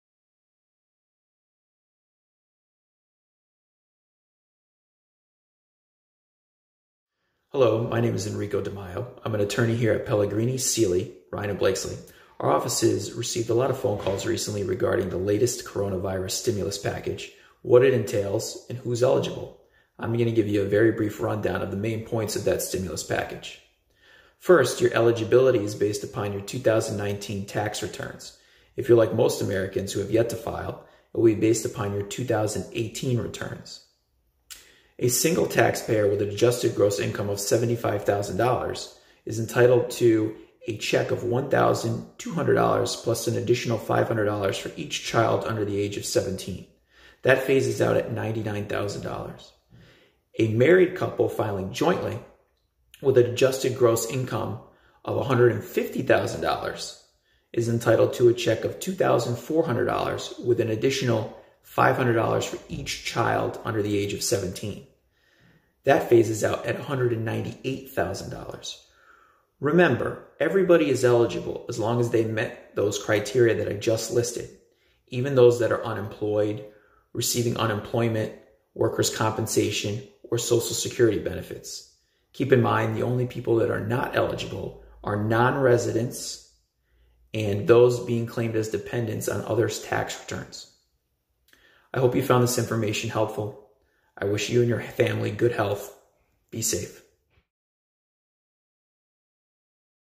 PSRB Stimulus Checks
PSRB Attorney Enrico De Maio breaks down how much stimulus money is available to you during this pandemic.